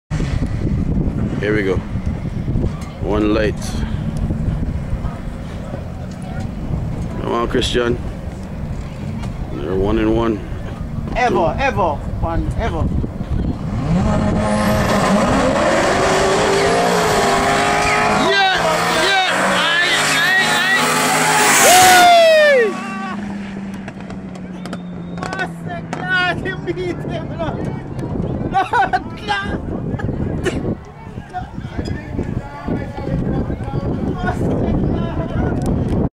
Christian vs Ian.MPG
March 27th, 2011  CIDRA - Cayman Islands Drag Racing Association first event.
Ian (Corvette) vs Marlon 'Christian' Ford (Evo3).  Ian ran a 6.8.  Christian ran a 7.0.  Christian had a better reaction time and crossed  the finish line first for the Win.